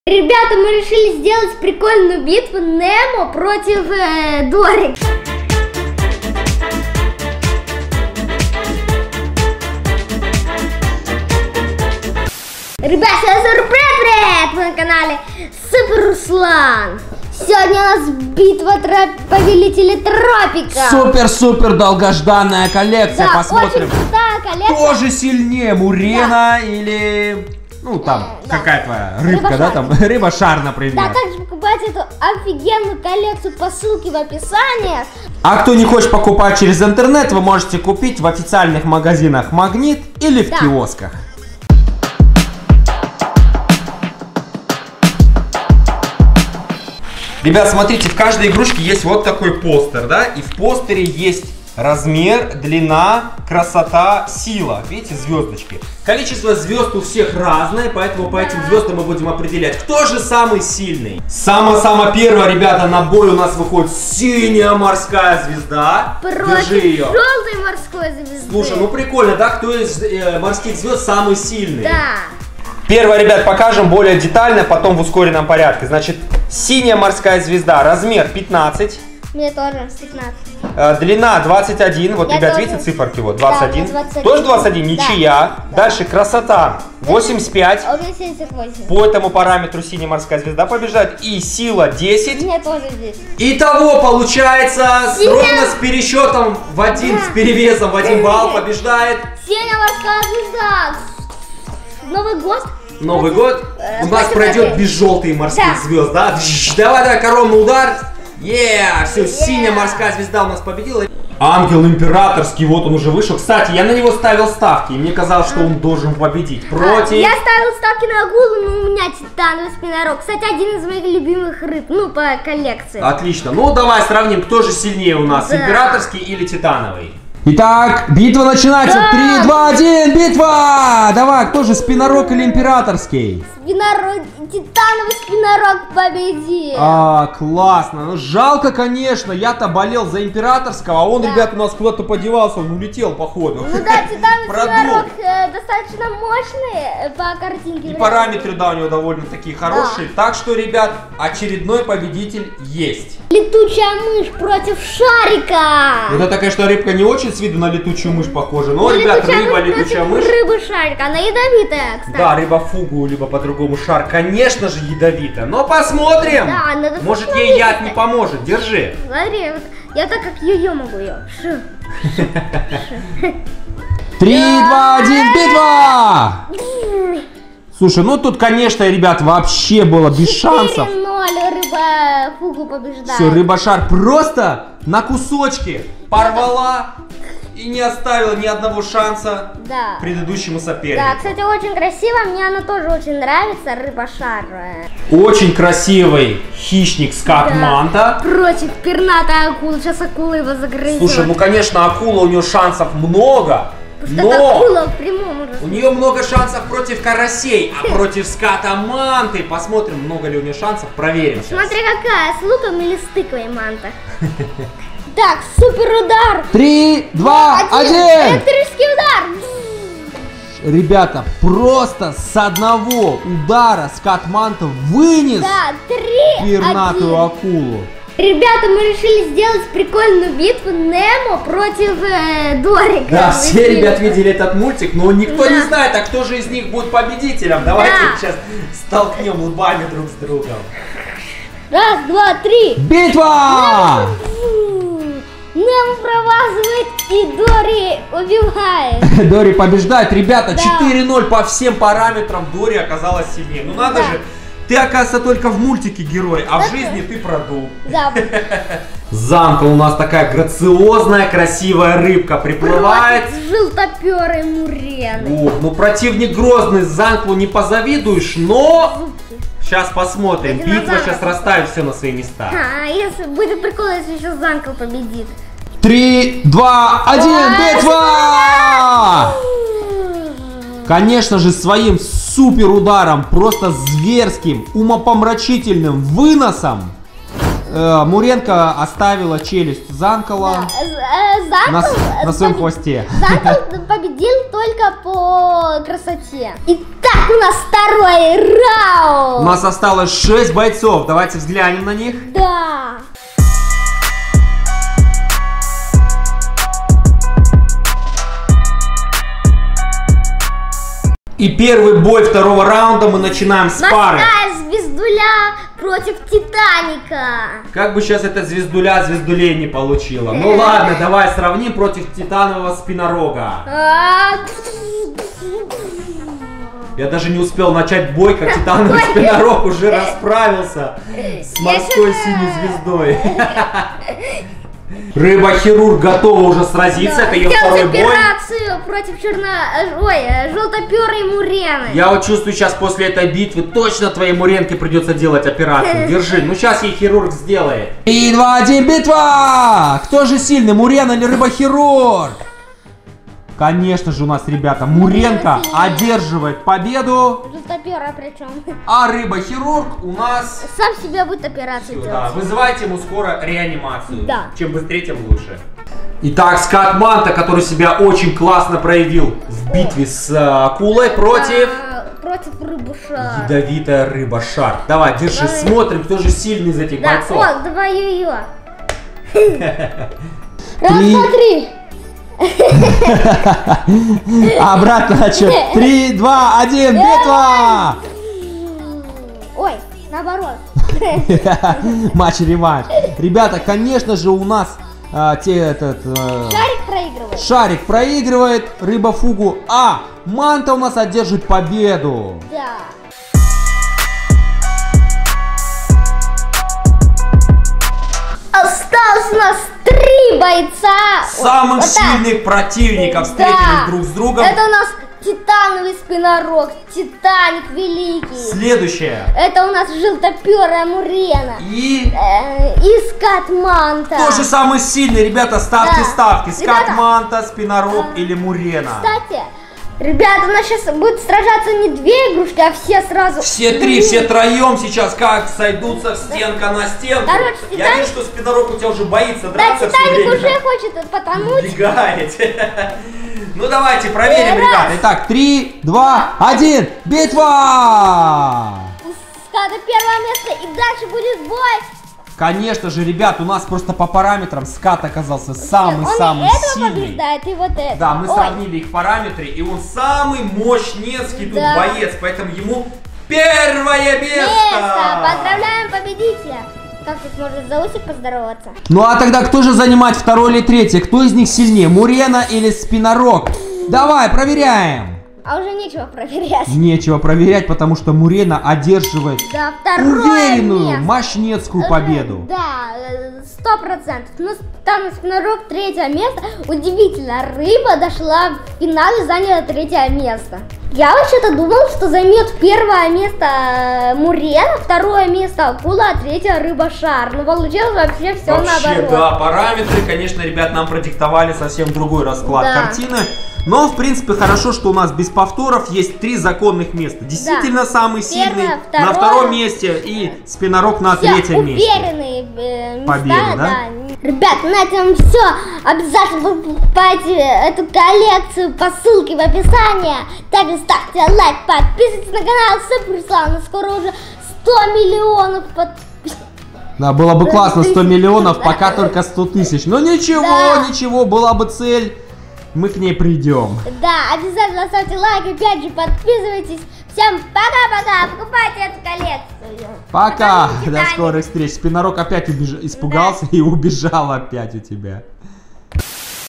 БИТВА Повелители ТРОПИКОВ Сбабам Новинка 2020
Повелители Тропиков или Повелители тропических рифов - это новая коллекция Animal Discovery от СБАБАМ (Sbabam), цель которой познакомить вас с обитателями кораллового рифа. 8 животных в коллекции сбабам обладают супер-эффектом: 2 могут менять свой цвет в горячей воде, 3 светятся в темное, 2 меняет цвет на солнце, а редкая Рыба-шар наполнена гелевыми шариками.
• ТОЧКИ ПРОДАЖ:
-КИОСКИ ПЕЧАТИ
-МАГНИТ
-WILDBERRIES

Смотрите видео на нашем детском канале Супер Руслан (Super Ruslan).

СПАСИБО, ЧТО СМОТРИТЕ НАШ КАНАЛ, МЫ ВАМ БЛАГОДАРНЫ!!!